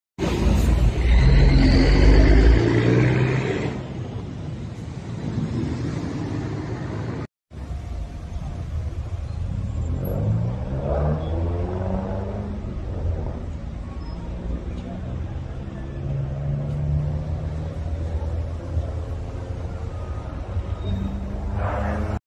Asalto a una taquería en el municipio de Apizaco
GraphosCc Tlx EsNoticia Asalto a una taquería en el municipio de Apizaco, deja como saldo un hombre herido de bala. Paramédicos de la Cruz Roja Mexicana acudieron al lugar. No hubo detenidos.